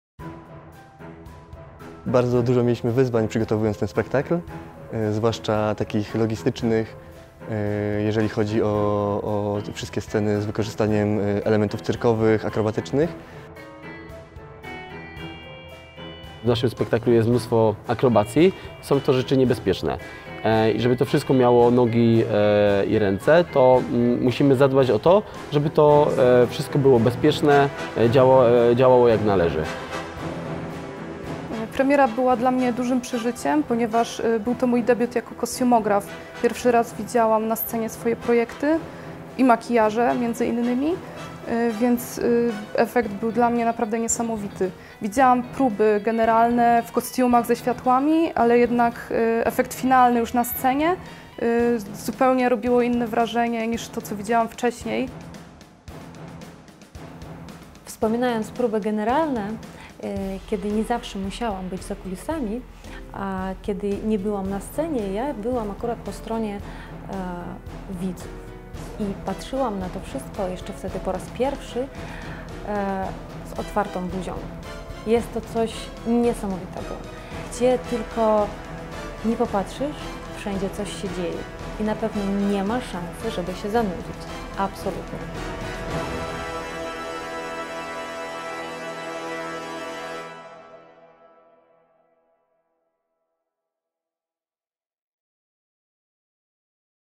Ach te Koty | Największe wyzwania Cz. 4 | Teatr Cortiqué
Co było największym wyzwaniem dla akrobatów? Co czuła jedna z artystek, przyglądając się przygotowaniom do show? Czy numery cyrkowe są niebezpieczne? Wszystkiego dowiecie się z zapowiedzi show Ach te Koty!

Zobacz, co o show mówią jego twórcy!

Choreograf i reżyser: Sandra Napierała
Choreograf, reżyser i odtwórca roli Szczura i Kota: Łukasz Banach
Odtwórca roli Szczura i Kota: Tomasz Krawczyk
Kostiumograf i asystent charakteryzatora: Zuzanna Malinowska
Wokal, odtwórczyni roli Mamy, Divy i Kocicy: Ksenia Shaushyshvili